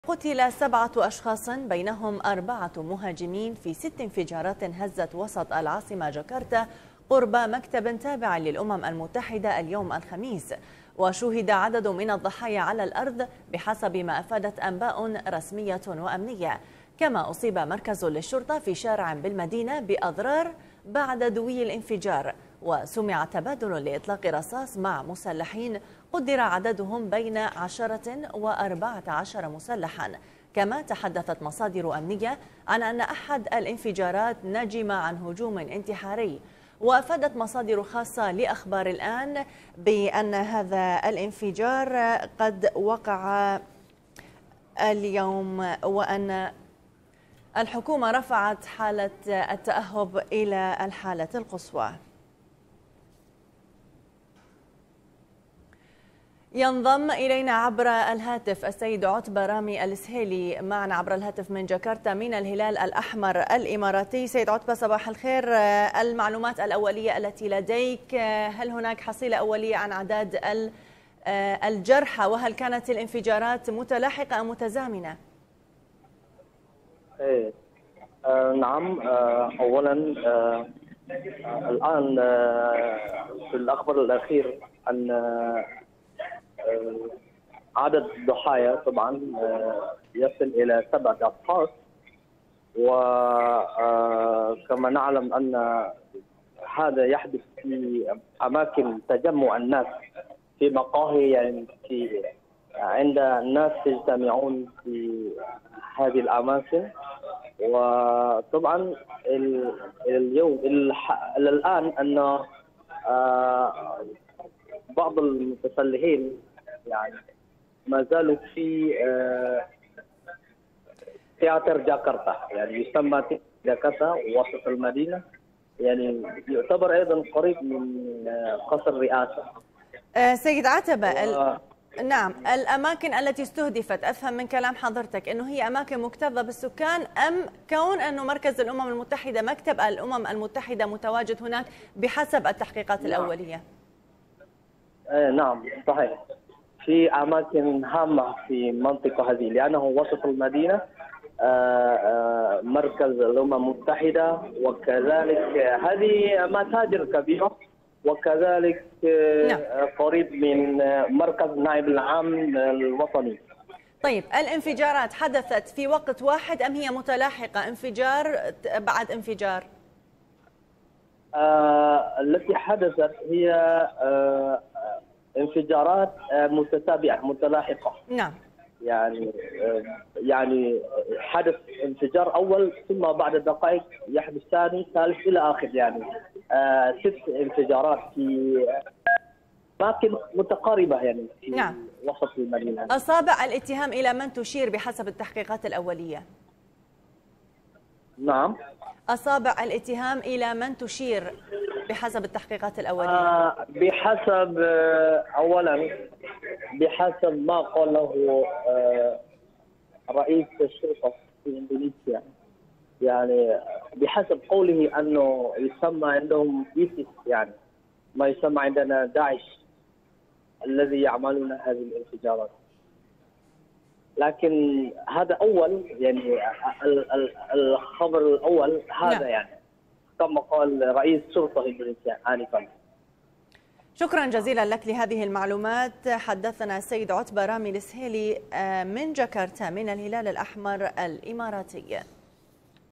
مقتل 7 أشخاص على الأقل في 6 انفجارات هزت العاصمة الاندونيسية جاكرتا
قتل 7  أشخاص بينهم 4 مهاجمين في 6 انفجارات هزت  وسط العاصمة الاندونيسية جاكرتا  قرب مكتب تابع للامم المتحدة اليوم الخميس وشوهد عدد من الضحايا على الارض،بحسب ما افادت انباء رسمية وأمنية .

كما اصيب مركز للشرطة في شارع بالمدينة باضرار بعد دوي الانفجار ، وسمع تبادل لإطلاق رصاص مع مسلحين ، قـُدر عددهم بين 10 و14 مسلحا  كما  تحدثت مصادر أمنية عن أن أحد الانفجارات نجم عن هجوم انتحاري. وافادت مصادر خاصة لأخبار الآن ان الحكومة رفعت حال الطوارئ الى القصوى